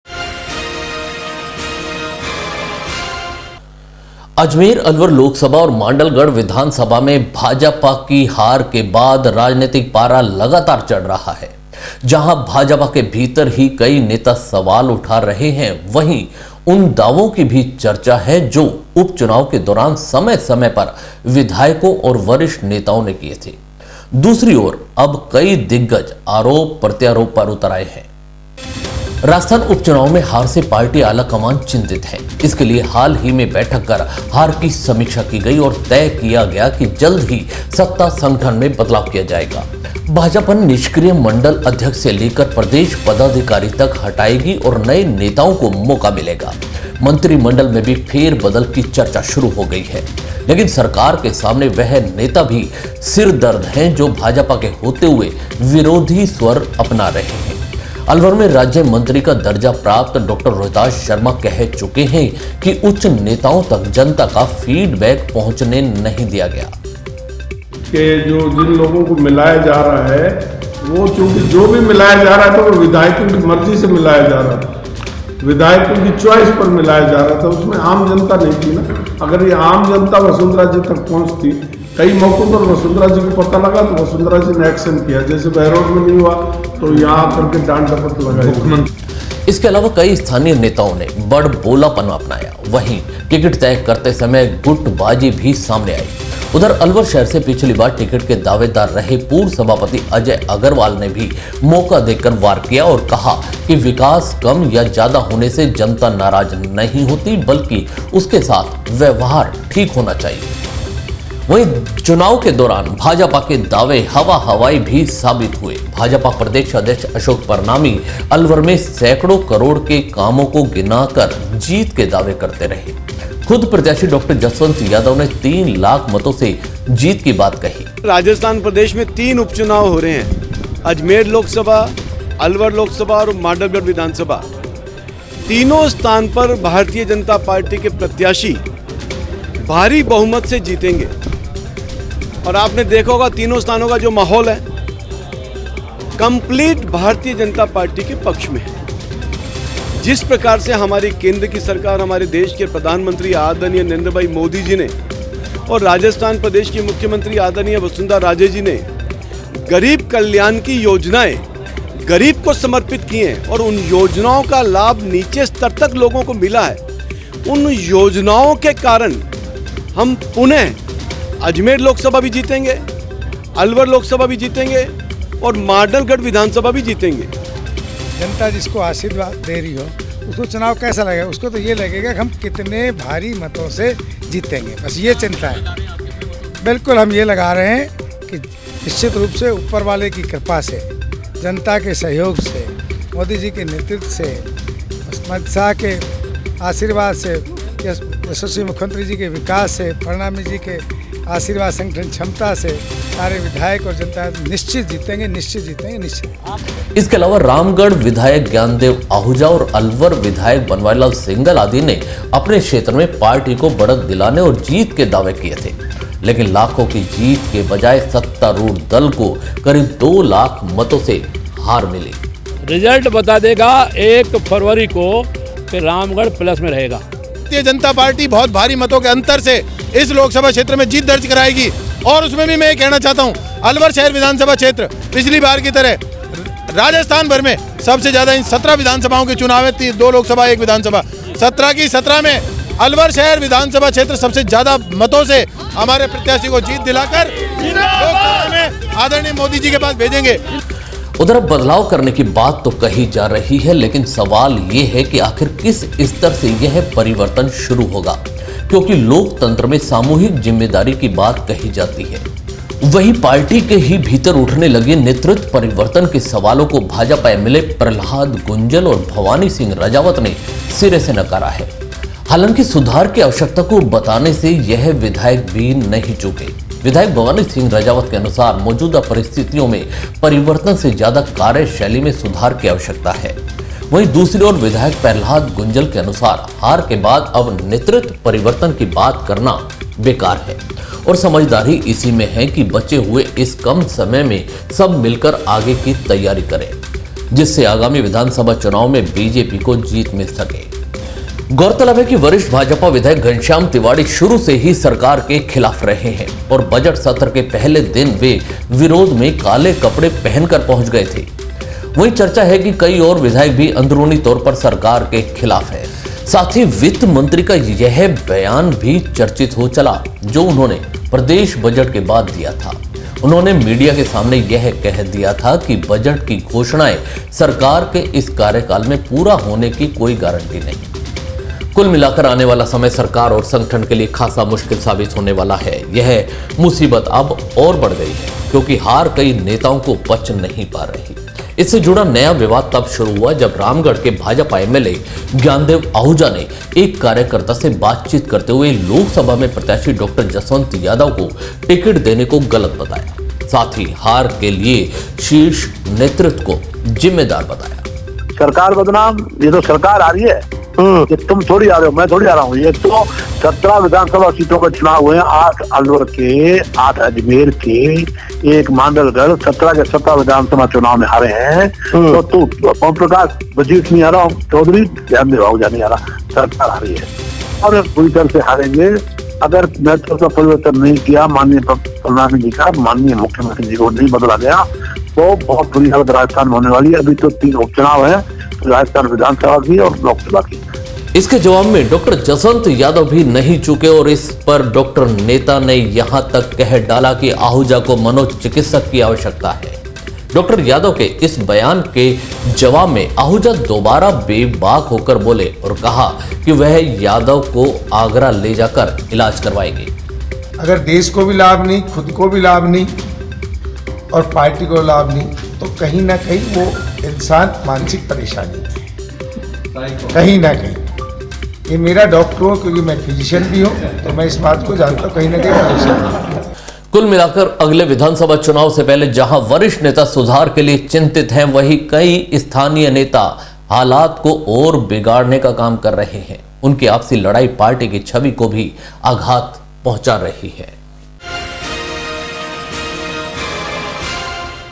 राजस्थान - उपचुनाव में हार ने दिग्गजों का बिगाड़ा बैलेंस, राजस्थान में छिड़ा बयानों का युद्ध
अजमेर, अलवर लोकसभा और मांडलगढ़ विधानसभा में बीजेपी की हार के बाद राजनीतिक पारा लगातार चढ़ रहा है। जहां, भाजपा के भीतर ही कई नेता सवाल उठा रहे हैं। वहीं, उन दावों की भी चर्चा हो रही है जो उपचुनाव के दौरान समय समय पर विधायकों और वरिष्ठ नेताओं ने किये थे। दूसरी ओर अब कई दिग्गज आरोप प्रत्यारोप पर उतर आए हैं।
राजस्थान उपचुनावों में हार से पार्टी आलाकमान चिंतित है। इसके लिए हाल ही में बैठकें कर हार की समीक्षा की गई और तय किया कि जल्द ही सत्ता-संगठन में बदलाव किया जाएगा। भाजपा निष्क्रिय मंडल अध्यक्ष से लेकर प्रदेश पदाधिकारी तक हटाएगी और नए नेताओं को मौका मिलेगा। मंत्रिमण्डल में भी फेरबदल की चर्चा शुरू हो गई है। लेकिन सरकार के सामने वह नेता भी सिरदर्द हैं जो भाजपा के होते हुए विरोधी स्वर अपना रहे हैं। अलवर में राज्यमंत्री का दर्जा प्राप्त डॉ रोहिताश्व शर्मा कह चुके हैं कि उच्च नेताओं तक जनता का फीडबैक पहुँचने नहीं दिया गया।
इसके अलावा कई स्थानीय नेताओं ने बड़बोलापन अपनाया। वहीं, टिकिट तय करते समय गुटबाजी भी सामने आई। उधर, अलवर शहर से पिछली बार टिकिट के दावेदार रहे पूर्व सभापति अजय अग्रवाल ने भी मौका देखकर वार किया और कहा कि विकास कम या ज्यादा होने से जनता नाराज नहीं होती बल्कि उसके साथ व्यवहार ठीक होना जरूरी है।
वहीं, चुनाव के दौरान भाजपा के दावे हवा हवाई ही साबित हुए। भाजपा प्रदेशाध्यक्ष अशोक परनामी अलवर में सैकड़ों करोड़ के कामो को गिना कर जीत के दावे करते रहे। खुद प्रत्याशी डॉ जसवंत यादव ने तीन लाख मतों से जीत की बात कही थी। इसके अलावा रामगढ़ विधायक ज्ञानदेव आहूजा और अलवर विधायक बनवारीलाल सिंघल आदि ने अपने क्षेत्र से पार्टी को बढ़त दिलाने और जीत के दावे किये थे। लेकिन लाखों की जीत के बजाये सत्तारूढ़ दल को करीब दो लाख मतों से हार नसीब हुई।
उधर, अब बदलाव करने की बात तो कही जा रही है लेकिन सवाल यह है कि आखिर किस स्तर से यह परिवर्तन शुरू होगा क्योंकि लोकतंत्र में सामूहिक जिम्मेदारी की बात कही जाती है। वहीं,, पार्टी के ही भीतर उठने लगे नेतृत्व परिवर्तन के सवालों को भाजपा एमएलए प्रहलाद गुंजल और भवानी सिंह राजावत ने सिरे से नकारा है। हांलाकि सुधार की जरुरत को बताने से यह विधायक भी नहीं चूके। विधायक भवानी सिंह राजावत के अनुसार मौजूदा परिस्थितियों में परिवर्तन से ज्यादा कार्यशैली में सुधार की जरूरत है। वहीं, दूसरी ओर, विधायक प्रहलाद गुंजल के अनुसार, हार के बाद अब नेतृत्व परिवर्तन की बात करना बेकार है और समझदारी इसी में है कि बचे हुए इस कम समय में सब मिलकर आगे की तैयारी करें जिससे आगामी विधानसभा चुनावों में बीजेपी को जीत मिल सके।
गौरतलब है कि वरिष्ठ भाजपा विधायक घनश्याम तिवाड़ी शुरु से ही सरकार के खिलाफ रहे हैं और बजट सत्र के पहले दिन वे विरोध में काले कपड़े पहन कर पहुँच गए थे। वहीं, चर्चा है कि कई और विधायक भी अंदरूनी तौर पर सरकार के खिलाफ हैं। साथ ही, वित्त मंत्री का यह बयान भी चर्चित हो चला है जो उन्होंने प्रदेश बजट के बाद दिया था। इसमें उन्होंने मीडिया के सामने यह कह दिया था कि बजट की घोषणाएं सरकार के इस कार्यकाल में पूरा होने की कोई गारंटी नहीं है।
कुल मिलाकर आने वाला समय सरकार और संगठन के लिए ख़ासा मुश्किल साबित होने वाला है। यह मुसीबत अब और बढ़ गई है। क्योंकि हार कई नेताओं को पच नहीं पा रही। इससे जुड़ा नया विवाद तब शुरू हुआ जब रामगढ़ के भाजपा एमएलए ज्ञानदेव आहूजा ने एक कार्यकर्ता से बातचीत करते हुए लोकसभा में प्रत्याशी डॉक्टर जसवंत यादव को टिकट देने को गलत ठहराया। साथ ही, हार के लिए शीर्ष नेतृत्व को जिम्मेदार बताया।
इसके जवाब में जसवंत यादव भी नहीं चूके और इस डॉक्टर नेता ने यहां तक कह डाला कि आहूजा को मनोचिकित्सक की जरूरत है। डॉक्टर यादव के इस बयान के जवाब में आहूजा दोबारा बेबाक होकर बोले और कहा है कि वह यादव का आगरा ले जाकर इलाज करवाएंगे।
कुल मिलाकर अगले विधानसभा चुनाव से पहले जहां वरिष्ठ नेता सुधार के लिए चिंतित हैं। वहीं, कई स्थानीय नेता हालात को और बिगाडऩे का काम कर रहे है। उनकी आपसी लड़ाई पार्टी की छवि को भी आघात पहुंचा रही है।